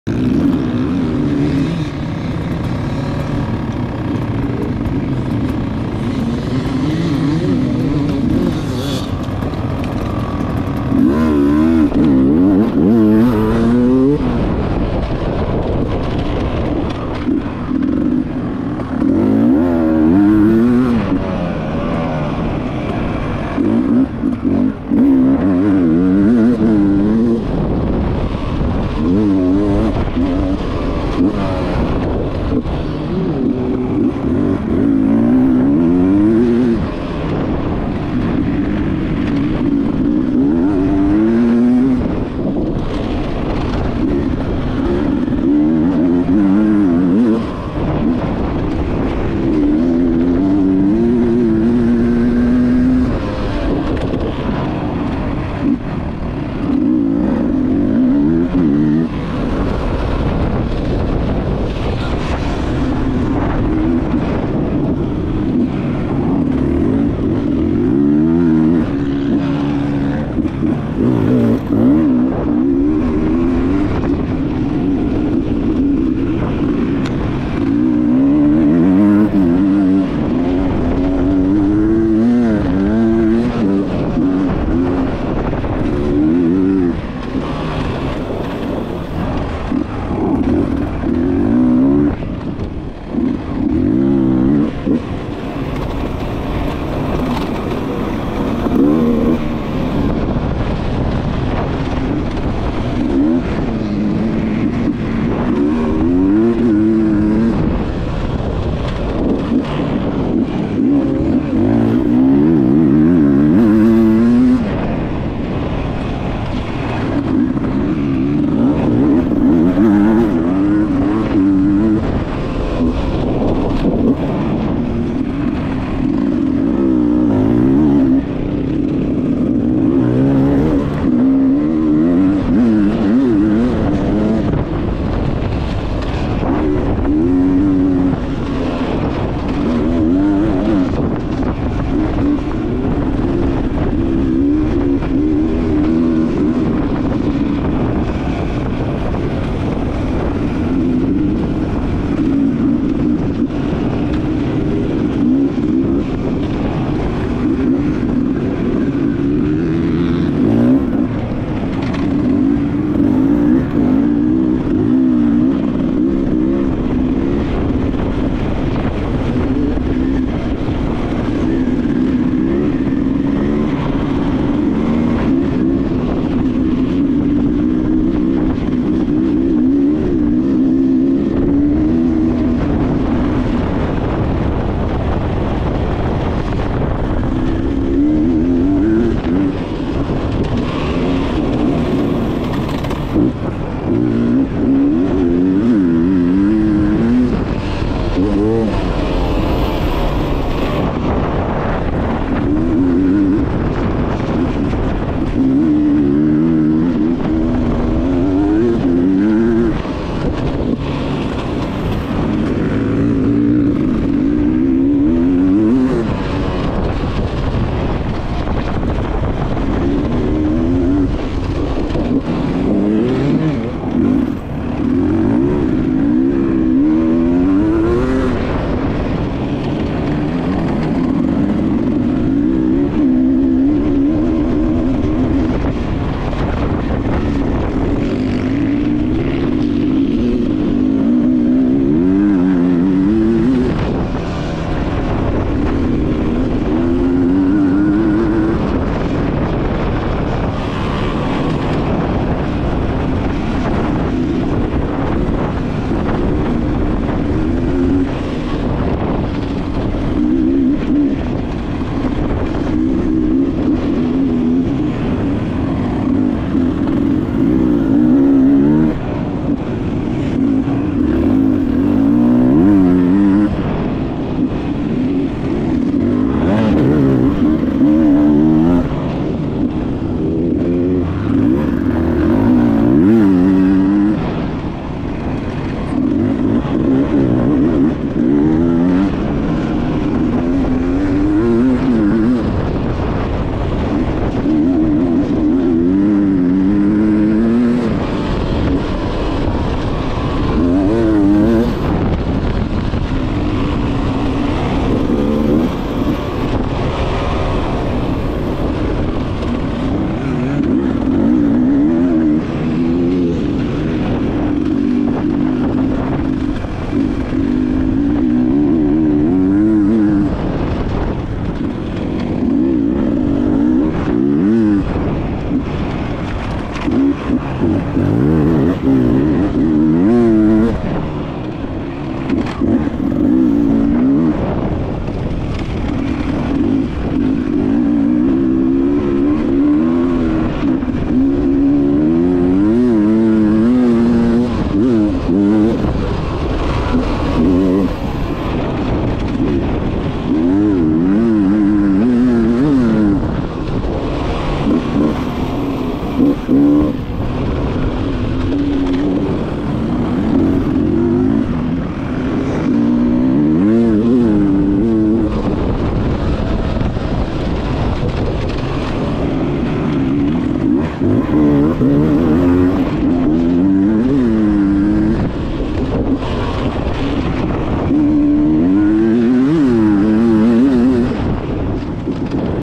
Cross Test Regionale Enduro Lazio/Abruzzo 18/06/2023
Cross Test regionale lazio abruzzo, nocciano 18/06/2023